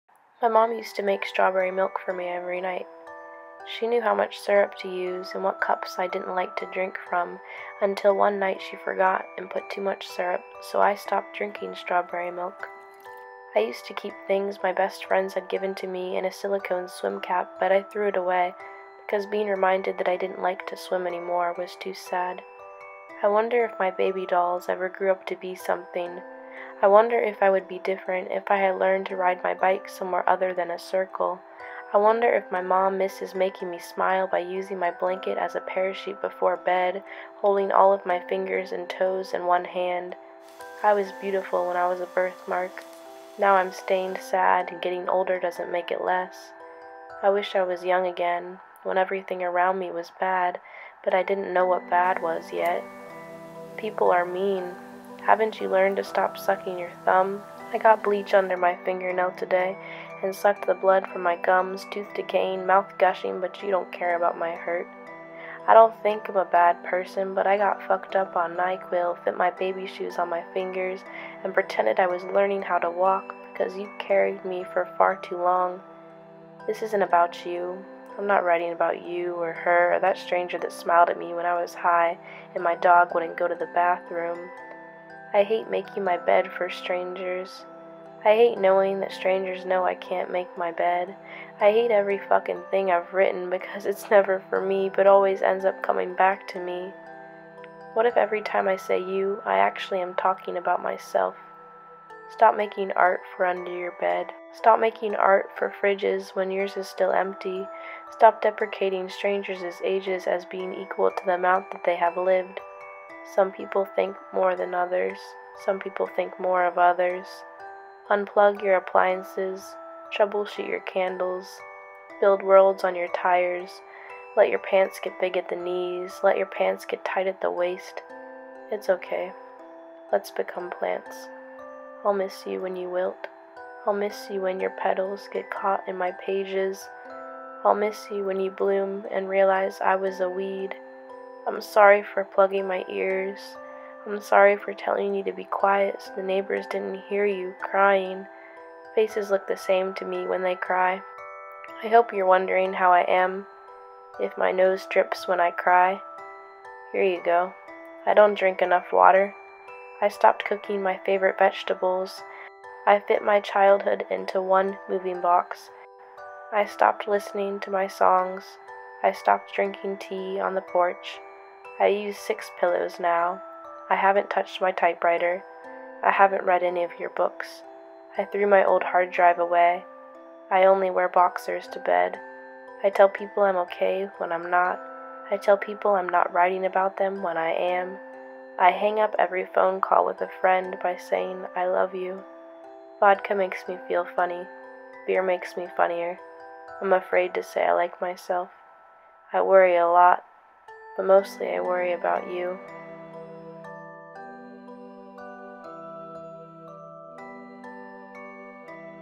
this isn't about you
//this is another visual piece to words i wrote.

text:
my mom used to make strawberry milk for me every night. she knew how much syrup to use and what cups i didn’t like to drink from, until one night she forgot and put too much syrup so i stopped drinking strawberry milk. i used to keep things my best friends had given to me in a silicone swim cap, but i threw it away because being reminded that i didn’t like to swim anymore was too sad. i wonder if my baby dolls ever grew up to be something. i wonder if i would be different if i had learned to ride my bike somewhere other than a circle. i wonder if my mom misses making me smile by using my blanket as a parachute before bed—holding all of my fingers and toes in one hand. i was beautiful when i was a birthmark. now i’m stained sad and getting older doesn’t make it less. i wish i was young again. when everything around me was bad, but i didn’t know what bad was yet.
people are mean. haven’t you learned to stop sucking your thumb?
i got bleach under my fingernail today and sucked the blood from my gums, tooth decaying, mouth gushing, but you don’t care about my hurt. i don’t think i’m a bad person, but i got fucked up on nyquil, fit my baby shoes on my fingers, and pretended i was learning how to walk, because you carried me for far too long. this isn’t about you. i’m not writing about you, or her, or that stranger that smiled at me when i was high and my dog wouldn’t go to the bathroom. i hate making my bed for strangers. i hate knowing that strangers know i can’t make my bed. i hate every fucking thing i’ve written, because it’s never for me, but always ends up coming back to me. what if every time i say “you” i actually am talking about myself? stop making art for under your bed. stop making art for fridges when yours is still empty.
stop deprecating strangers’ ages as being equal to the amount that they have lived. some people think more than others. some people think more of others. unplug your appliances. troubleshoot your candles. build worlds on your tires. let your pants get big at the knees. let your pants get tight at the waist. it’s ok. let’s become plants. i’ll miss you when you wilt. i’ll miss you when your petals get caught in my pages. i’ll miss you when you bloom and realize i was a weed.
i’m sorry for plugging my ears. i’m sorry for telling you to be quiet so the neighbors didn’t hear you (crying). faces look the same to me when they cry. i hope you’re wondering how i am. if my nose drips when i cry.
i don’t drink enough water. i stopped cooking my favorite vegetables. i fit my childhood into one moving box. i stopped listening to my songs. i stopped drinking tea on the porch. i use six pillows now. i haven’t touched my typewriter. i haven’t read any of your books. i threw my old hard drive away. i only wear boxers to bed. i tell people i’m okay when i’m not. i tell people i’m not writing about them when i am. i hang up every phone call with a friend by saying “i love you.” vodka makes me feel funny. beer makes me funnier. i’m afraid to say i like myself. i worry a lot. but mostly i worry about you.

love,
lauren